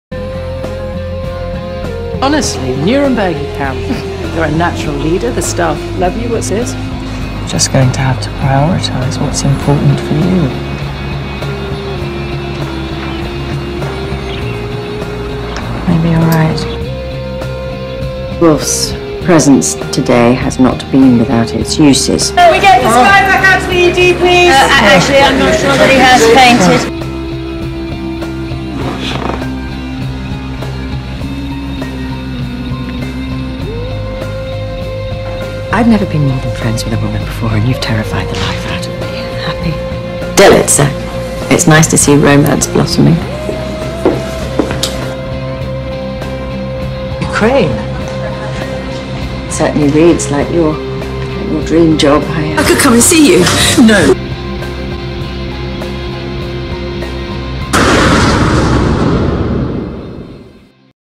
Bernie and Serena: What Now?
Here's a little trailer I made up for next week's probable sapphic angst fest of an episode. I hope you all in enjoy it